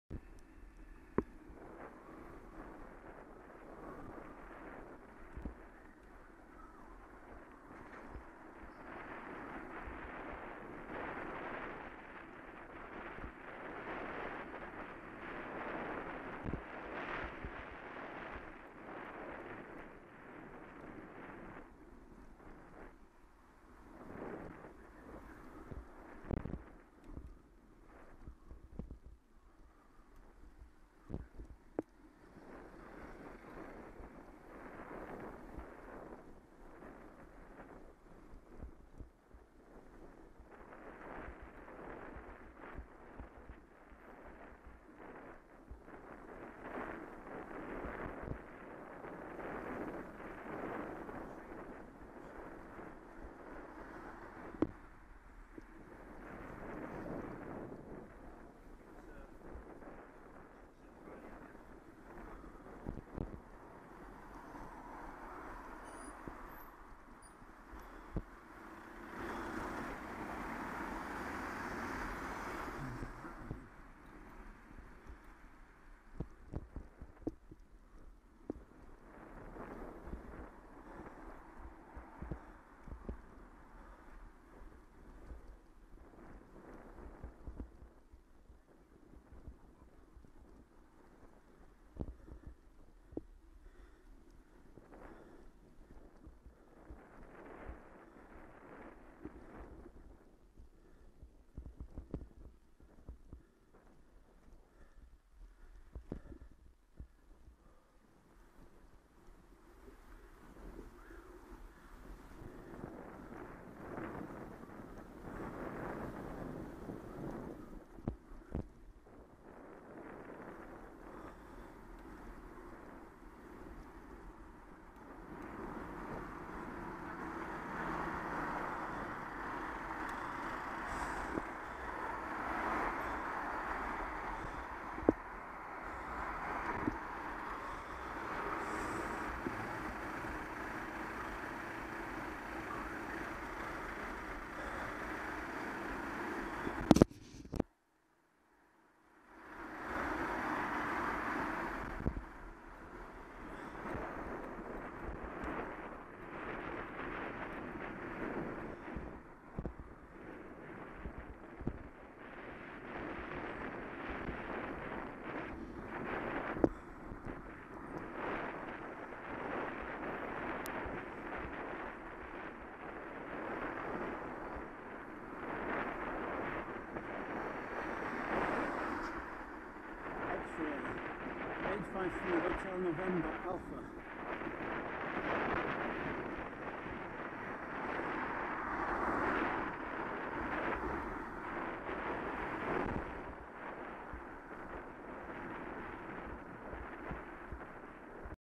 Deansgate to Higher Bridge Street
This route was proposed as part of a Bee Network scheme, but half of it, including a very hostile junction, was cut out by the council because of fears over affecting motor traffic flows. (In fact the statement was that traffic flows need to be modelled before including a junction in a proposal.)